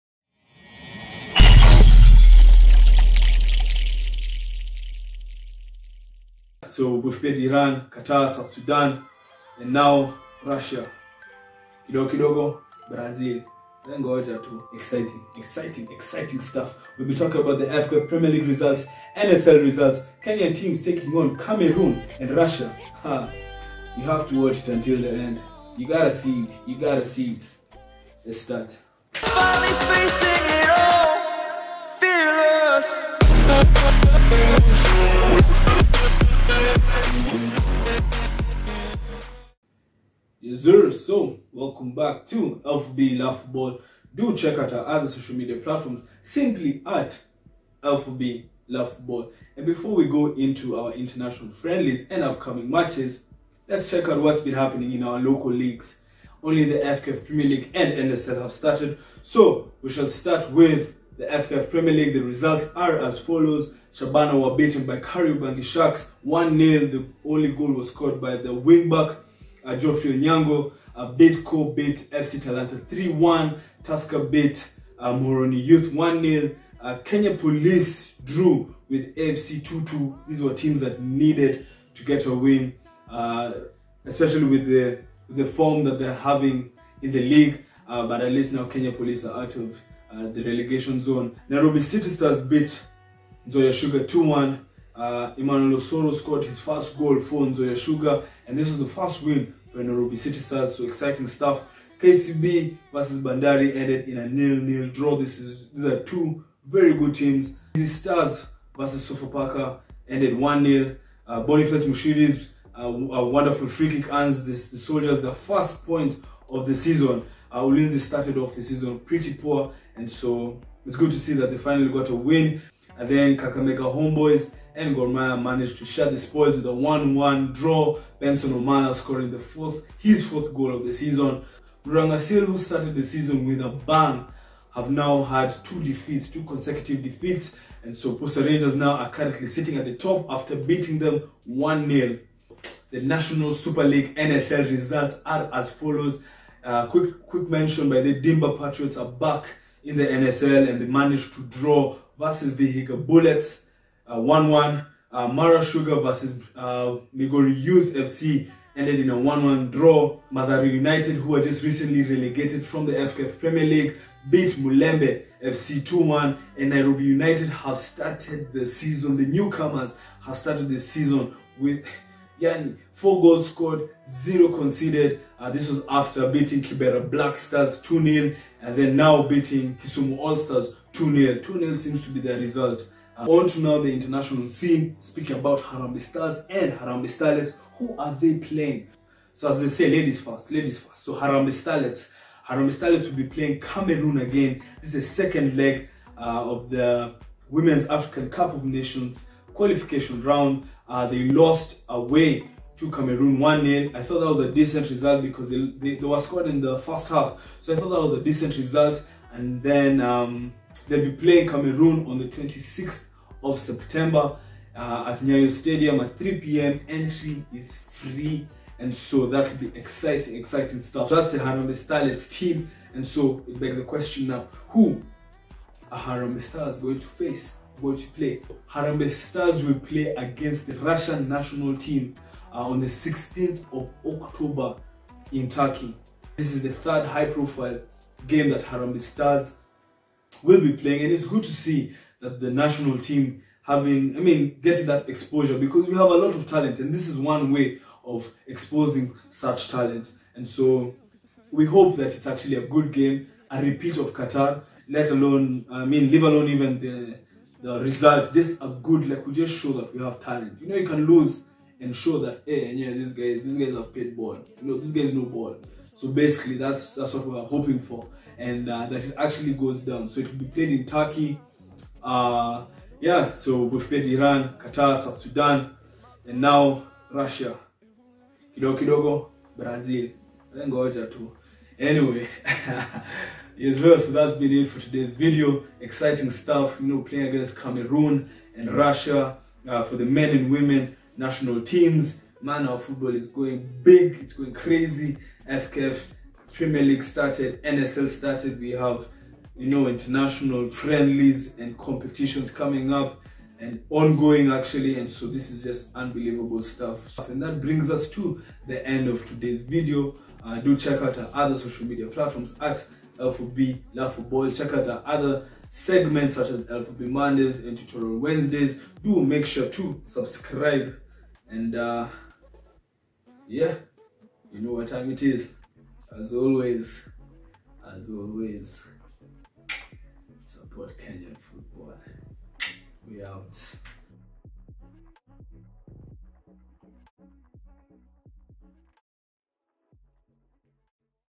Harambee Stars to Face Russia & Starlets to Play Cameroon | FKFPL & NSL Weekend Results.
Kenya to play Russia? Really? Unexpected lowkey announcement as Kenya may face Russia in an international friendly in Turkey scheduled for 16th October. This would mean a series of high profile games after the likes of Iran and Qatar. The Kenyan women's team takes on Cameroon in the 2nd leg of the Women's African Cup of Nations Qualification rounds where they trail by one goal after losing 1-0 away. We also review some of the FKFPL and NSL results from the past weekend.
Remember to Support Kenyan Football
Information retrieved from Kevin Teya and football ke.